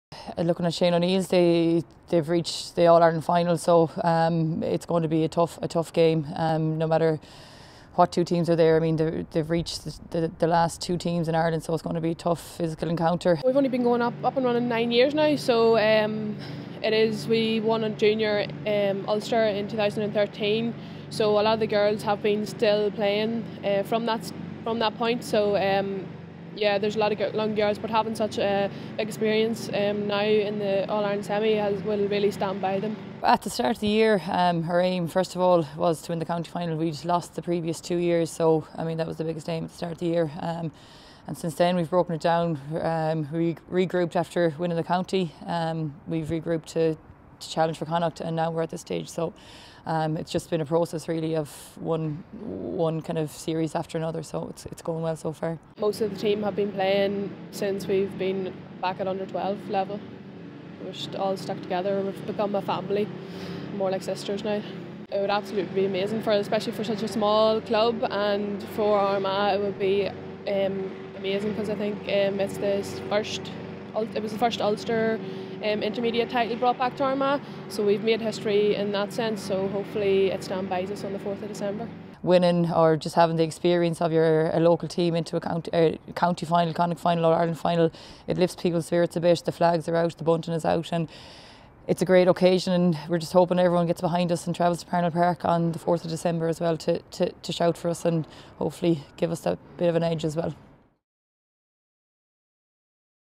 Shane O'Neills v Annaghdown preview
Preview for the 2016 All Ireland Intermediate Club Final between Shane O'Neills (Armagh) and Annaghdown (Galway).